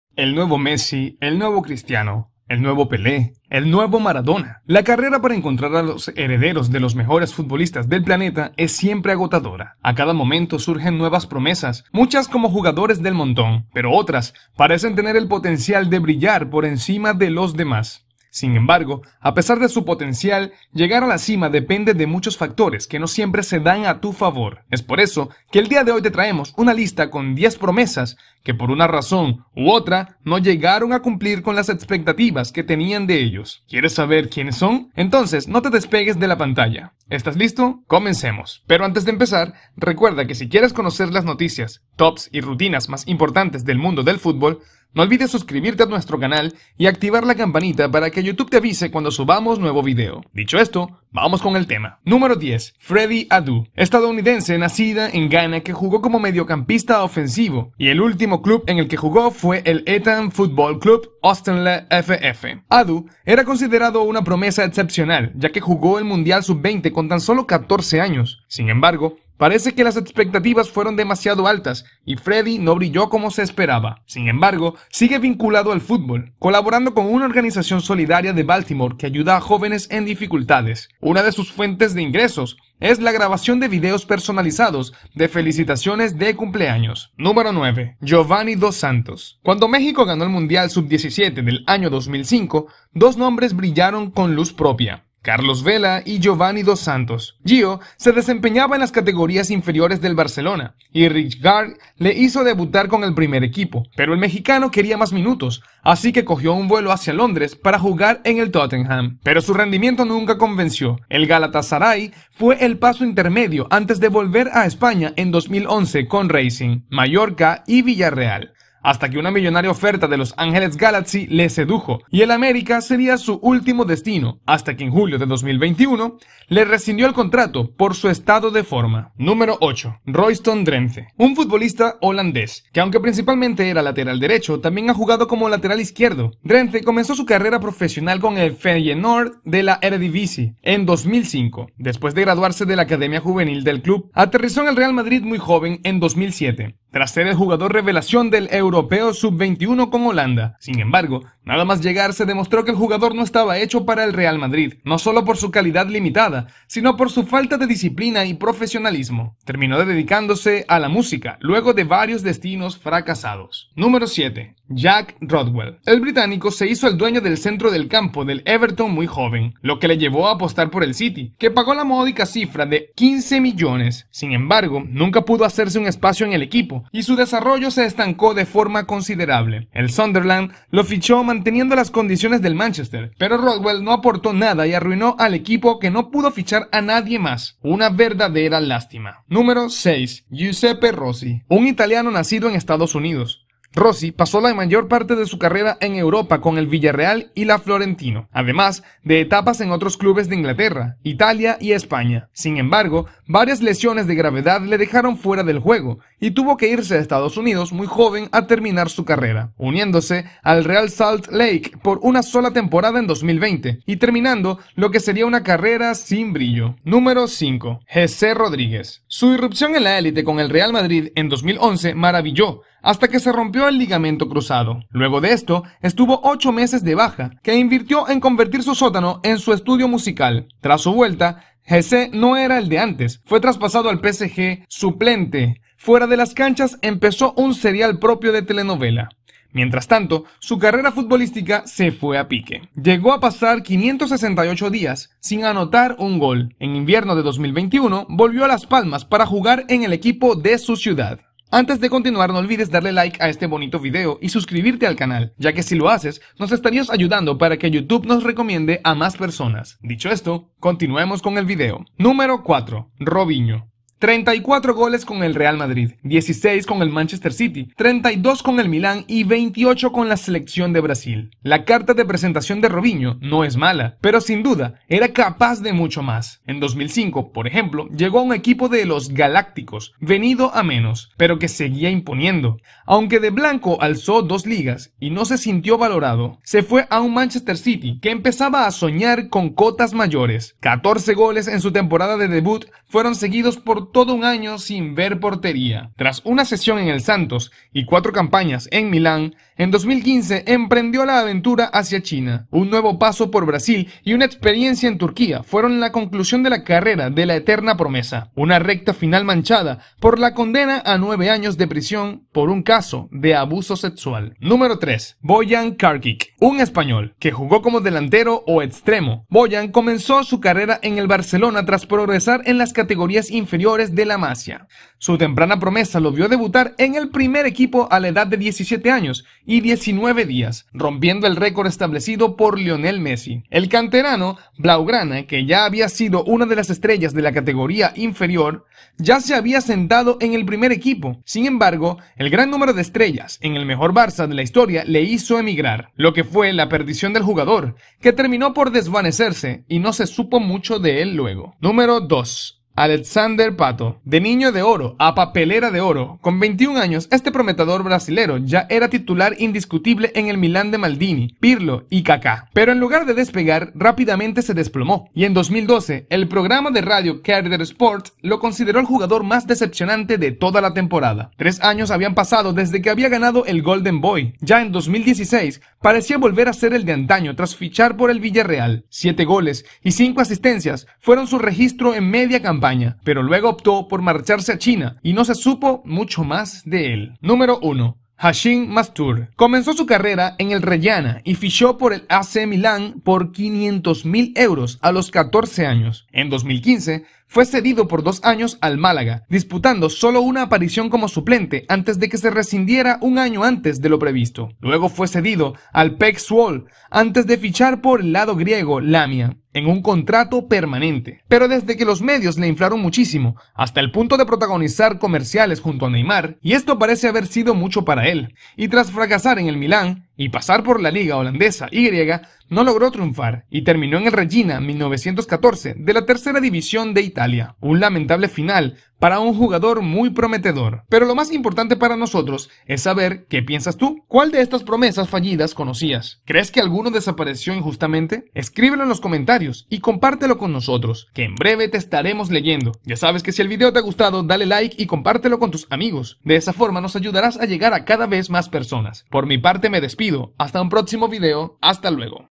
10 PROMESAS del FUTBOL que FRACASARON
Todos conocemos a algún amigo que podría haber llegado a ser profesional pero fracasó en el intento. Pero hay jugadores de fútbol de los que se esperaba el estrellato pero fracasaron estrepitosamente.
Hoy, en Reyes del Esférico, veremos: 1o promesas del futbol que fracasaron

DENTRO VÍDEO!